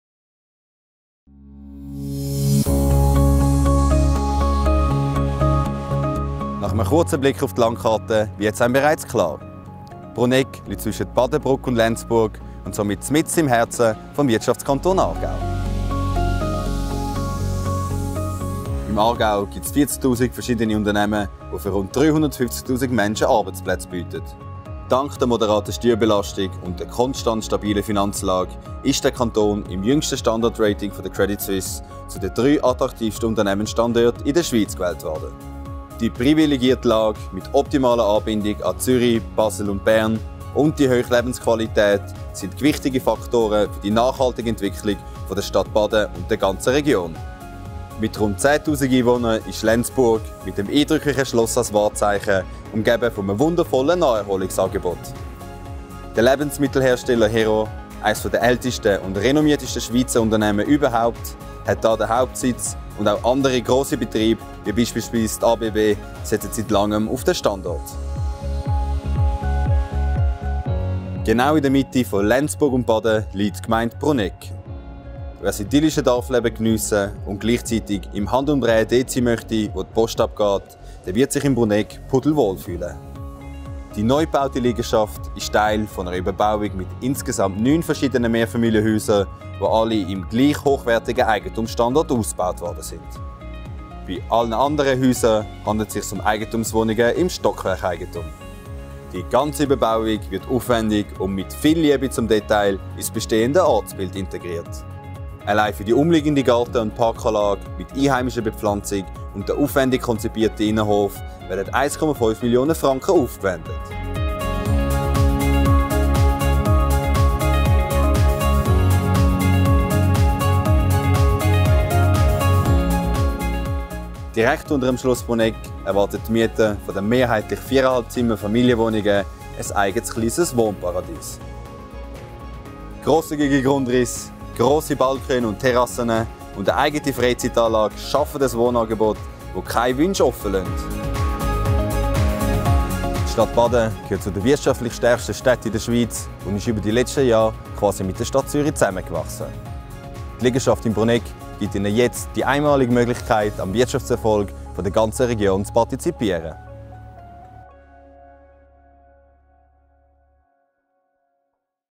Renditeliegenschaft in Brunegg AG – Crowdhouse Investieren
Dieses Mehrfamilienhaus wurde im Oktober 2017 von 28 Miteigentümern finanziert.